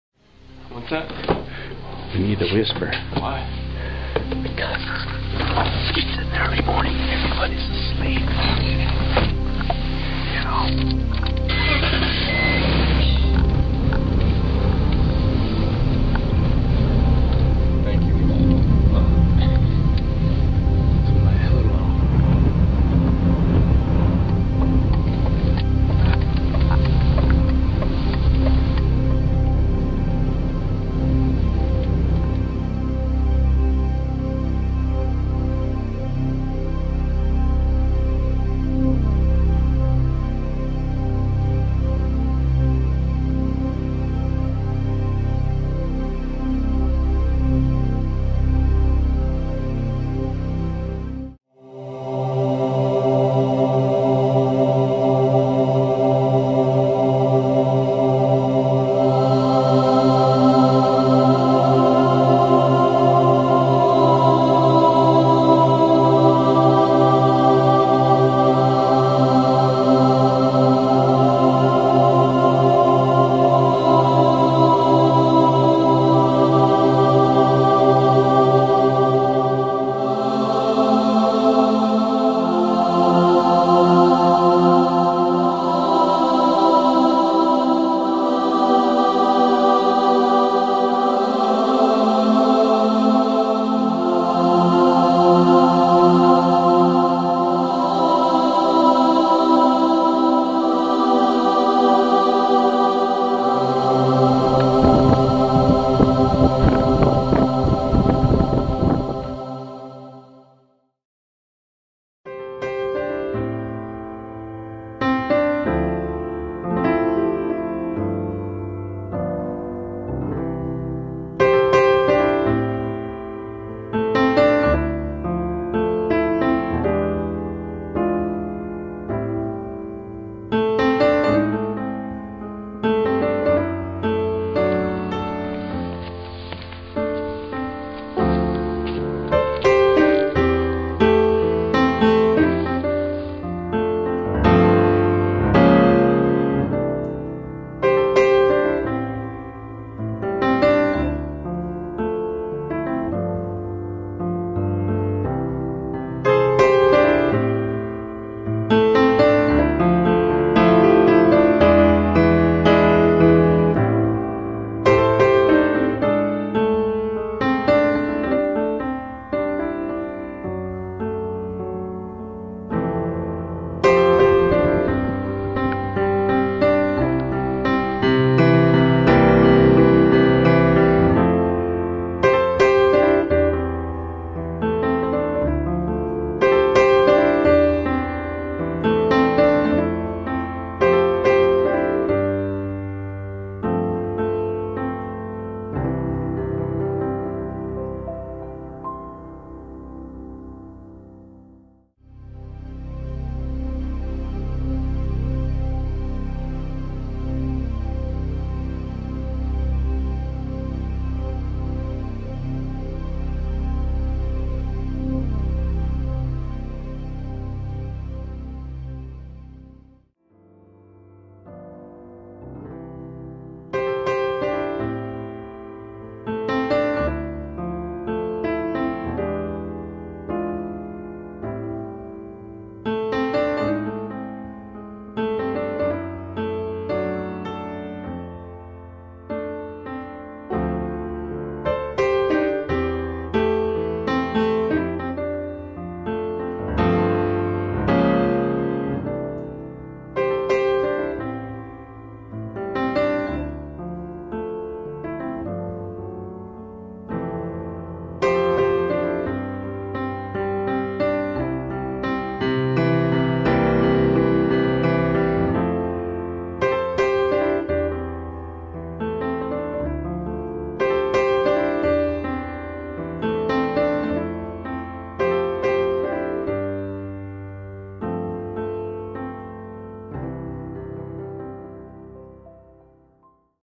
Stonewall Peak Sunrise, San Diego
My son and I went to Stonewall Peak, Cuyamaca, California to watch the sunrise on Saturday, June 14, 2014.

I used iMovie to create this video from the pictures and video we shot during the hike. The music is part of the iMovie audio selections. Anybody can do this. This is my second video using the program.